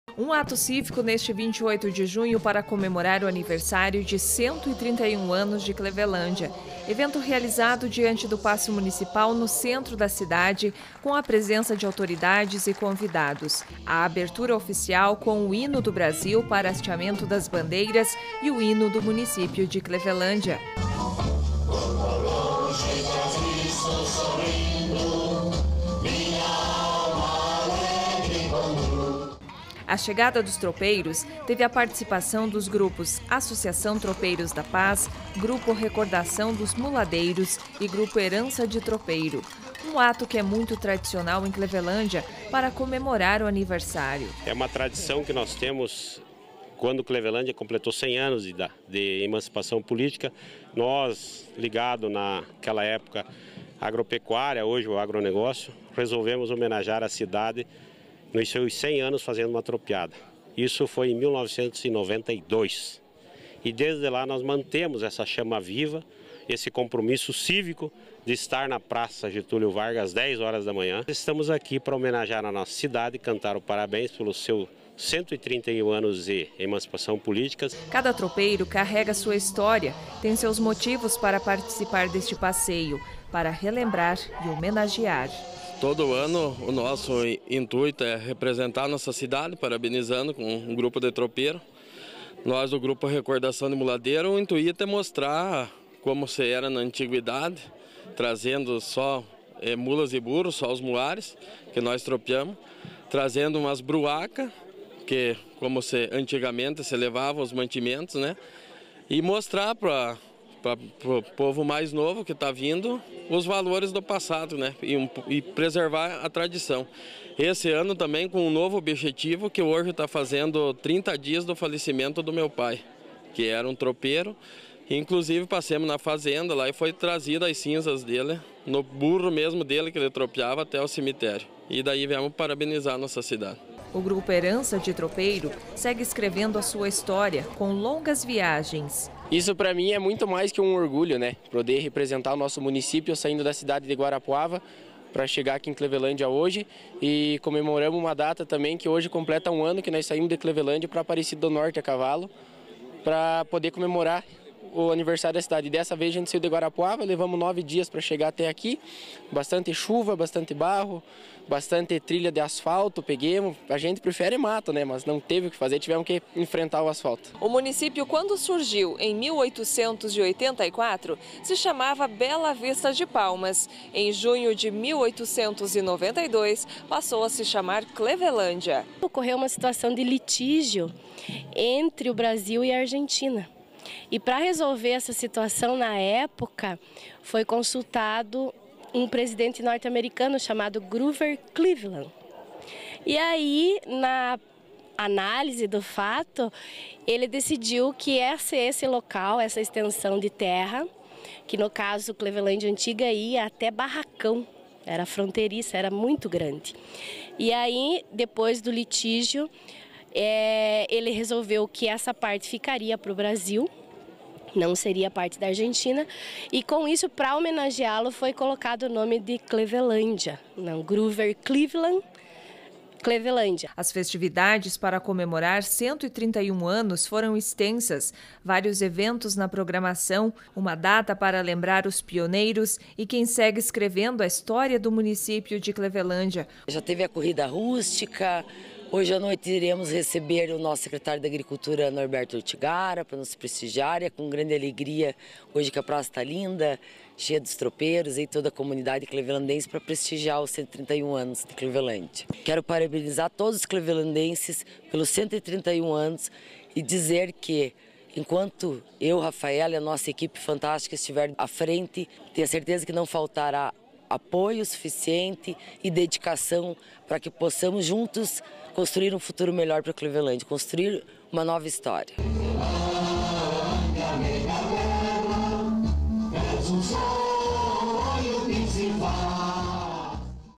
Clevelândia comemora 131 anos
Um ato cívico foi realizado diante do passo municipal para comemorar a data.